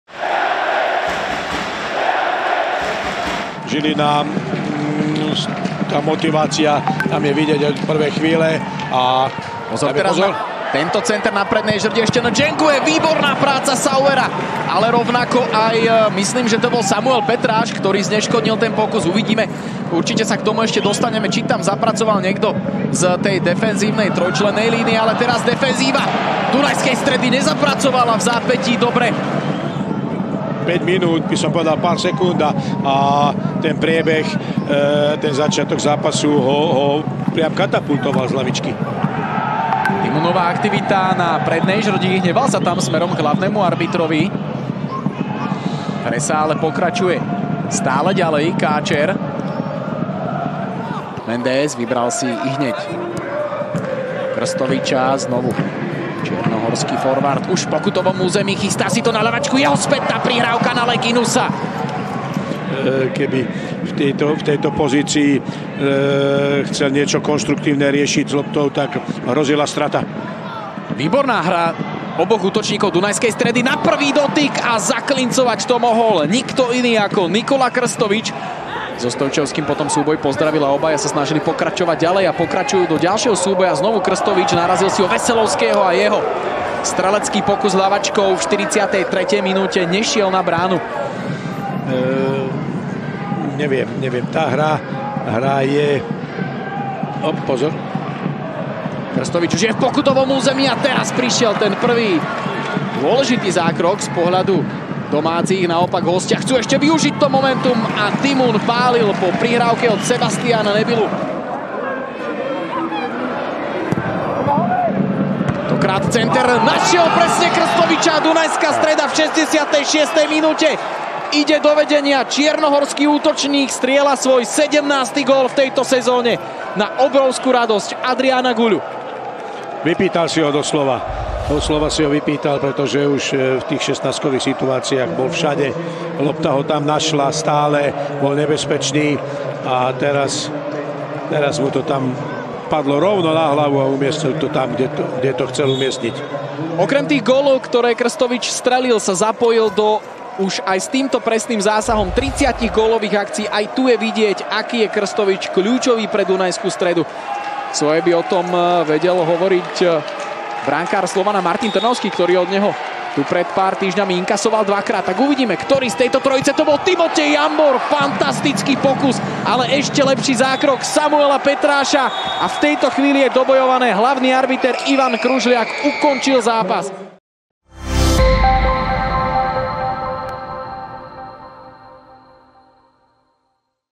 MŠK Žilina - DAC 1904 0:1 (0:0)
DAC: Petráš - Kružliak (K), Muhamedbegovic, Brunetti (76. Davis) - Leginus, Veselovský (84. Blackman), Dimun, Káčer (76. Riquelme), Mendez - Krstović, Niarchos (57. Nebyla)
Žilina: Belko - Minárik, Stojchevski, Minárik (K) - Rusnák (74. Javorček), Gidi, Sauer (67. Iľko), Bari - Kaprálik, Ďuriš (74. Essomba), Galčík (67. Jambor)